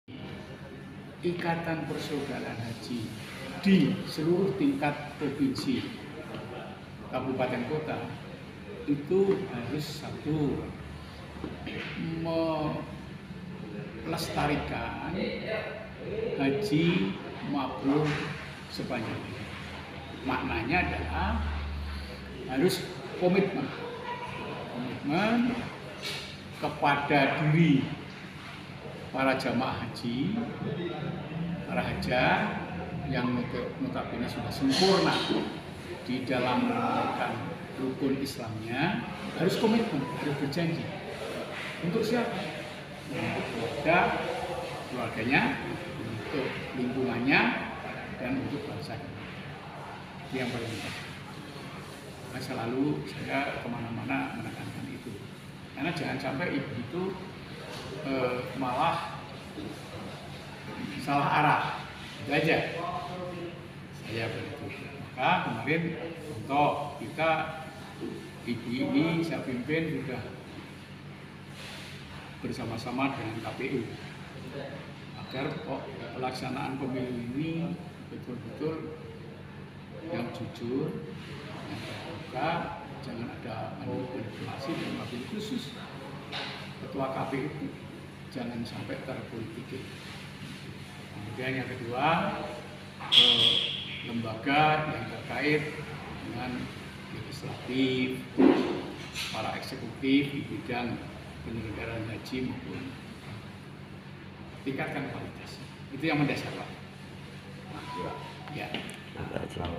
Ketua Umum IPHI Pusat Bapak Dr H.Erman Suparno M.Si.
Bali - Lokadewatanews - Tim Media Liputan Terkait Acara Ketua umum IPHI pusat .bapak Dr H.Erman Suparno Didampingi Ketua Pembina /atau Advokat nya H. Hari Wantono,S.H., M.H. jln Perademak Denpasar Bali. Minggu (22/10/2023).